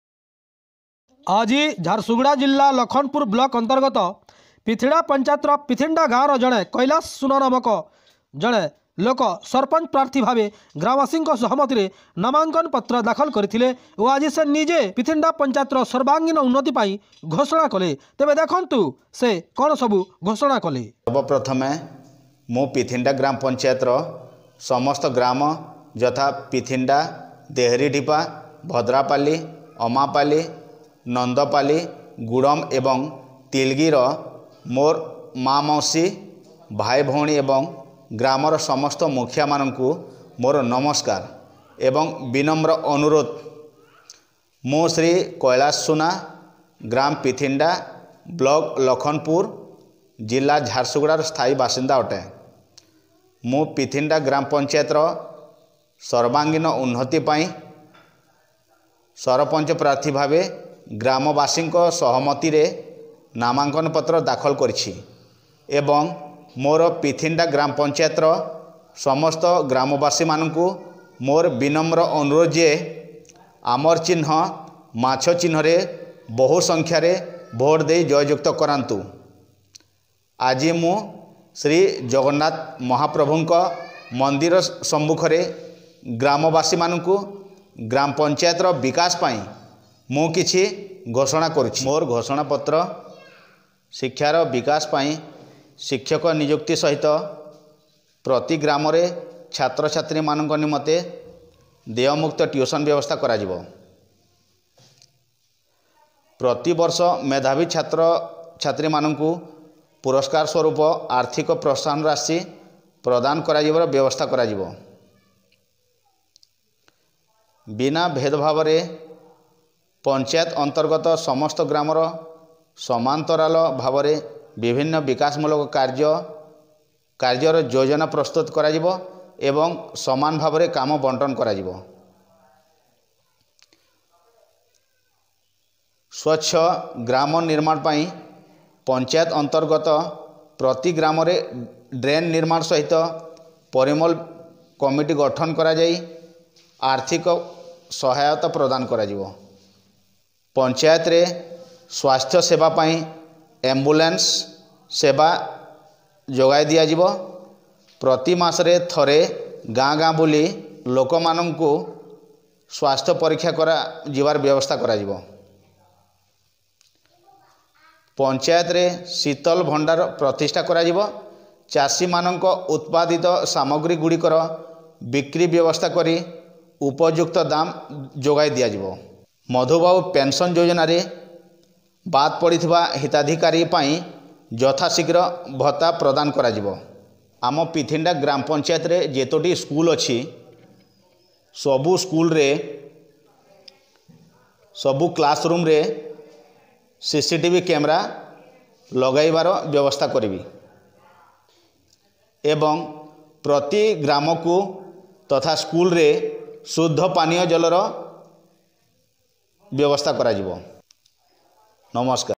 ପିଥିଣ୍ଡା ଗାଁ ର କୈଳାଶ ସୁନା ପଞ୍ଚାୟତି ଭୋଟ ନହେବା ଆଗରୁ ଘୋଷଣା କଲେ କଣ ସବୁ କାମ କରିବେ
ପିଥିଣ୍ଡା ଗାଁ ର କୈଳାଶ ସୁନା ପଞ୍ଚାୟତି ଭୋଟ ନହେବା ଆଗରୁ ଘୋଷଣା କଲେ କଣ ସବୁ କାମ କରିବେ  l  ସ୍କୁଲ ରେ ପାନୀୟ ଜଳ, ସି ସି ଟିଭି କ୍ୟାମରା, ଶୀତଲଭଣ୍ଡାର, ଆହୁରି ଅନେକ ସମସ୍ୟା